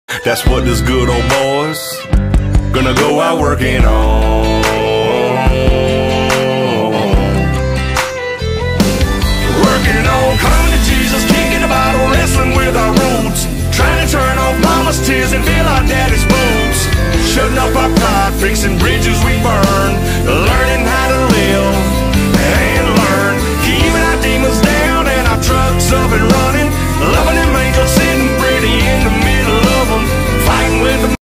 Asphalt pothole filling - Pittsburgh, PA
Asphalt pothole filling with @kminternational6966 hotbox.